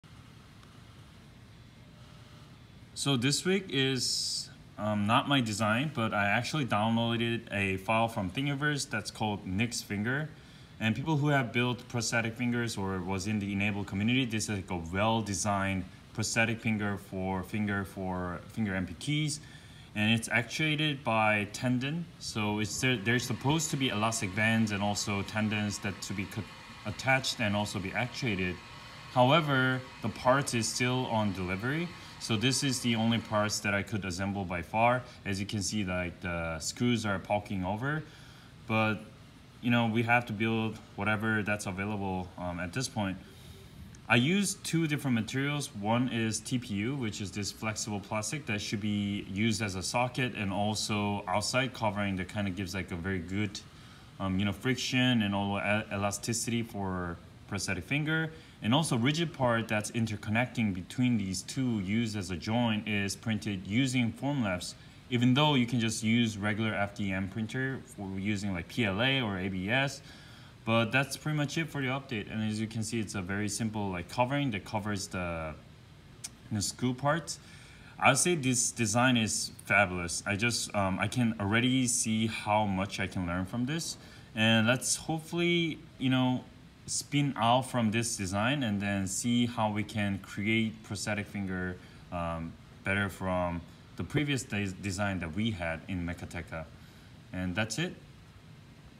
Printing and assembling Knick's finger
This week is a learning week.
One of the most popular prosthetic finger design is by @nsbrookins  called 'Knick's finger'.

It's a tendon-driven system, that is composed of rigid and soft materials.

I printed out using this design in 100% scale.